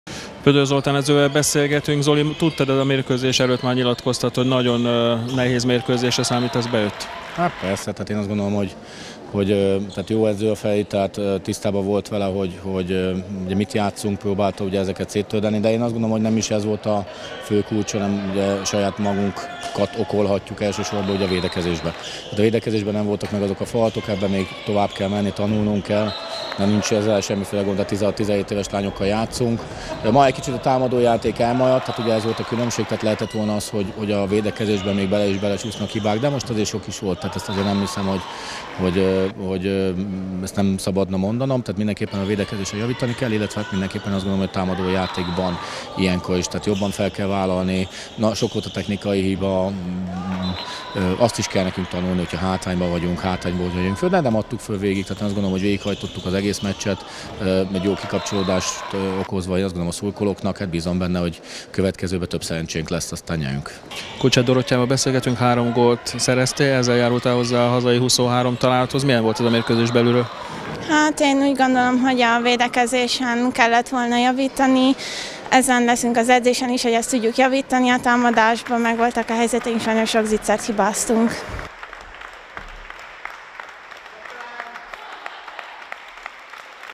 SZKKA - Mohács NB1/B-s mérkőzés értékelés
Edző és játékos értékelés a mérkőzés után.